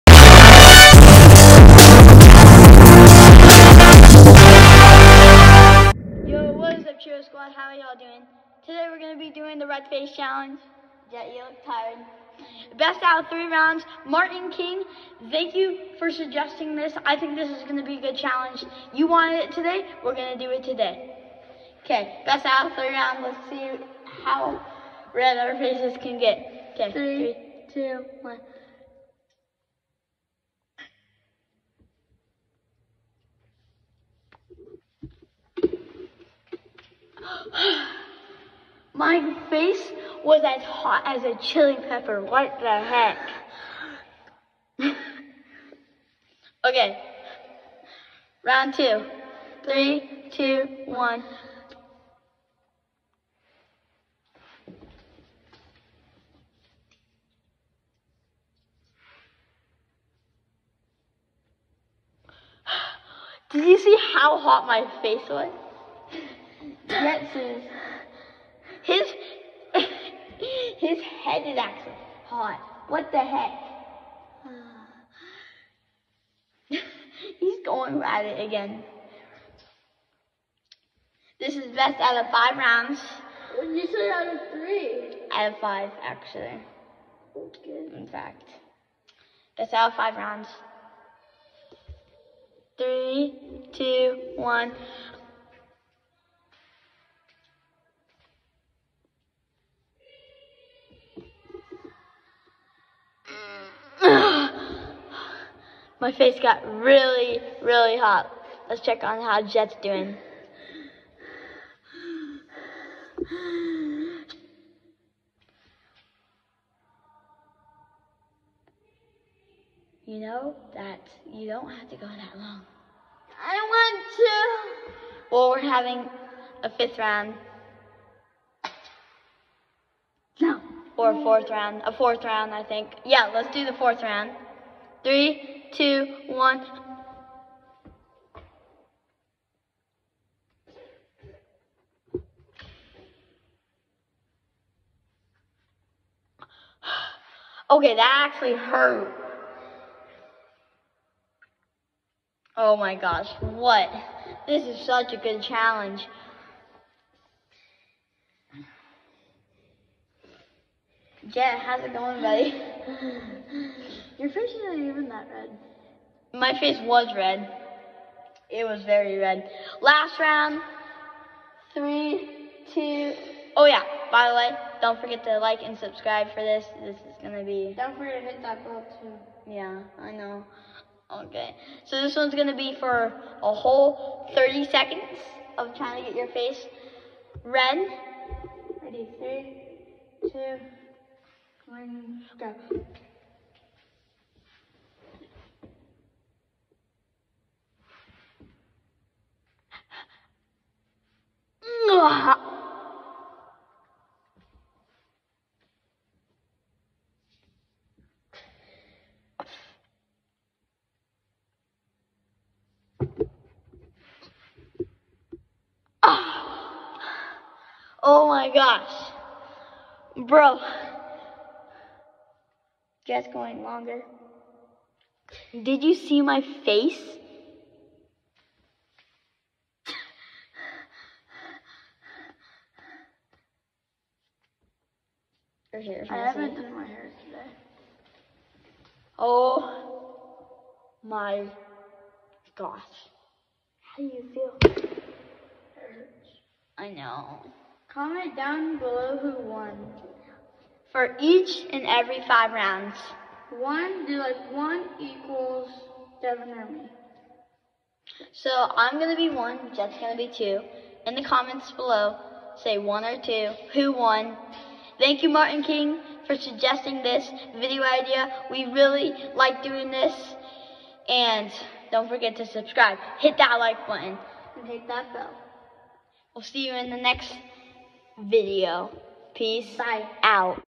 Red face challenge. ( to dang hot 🥵)
how red can your face can get, best out of 5 rounds.